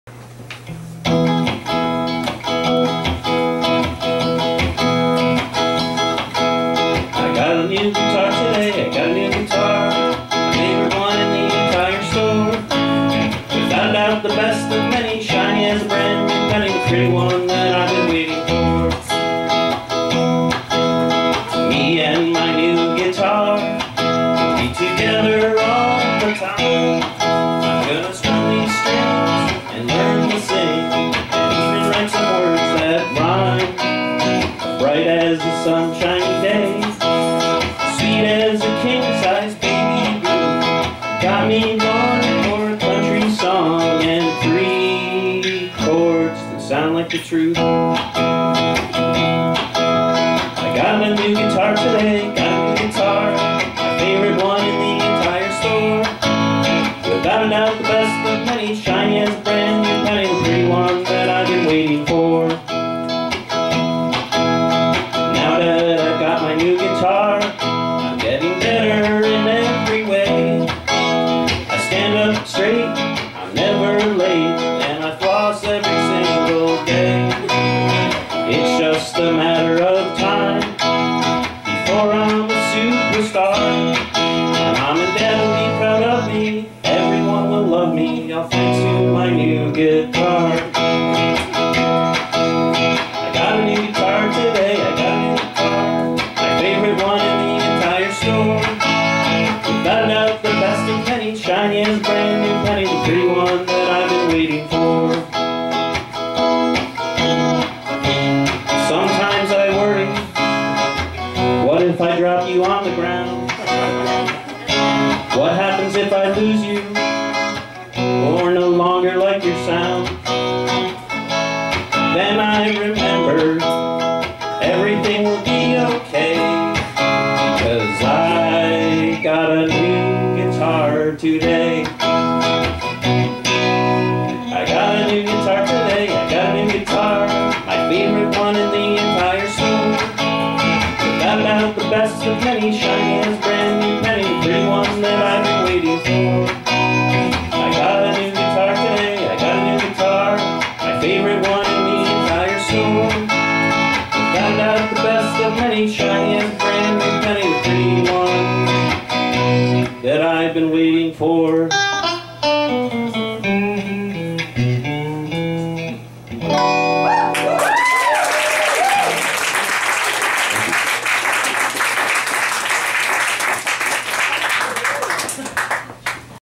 Larry plays an original song
Park Slope Voice Lessons - Winter Concert:
Saturday, December 14, 2013 at Jalopy Theatre.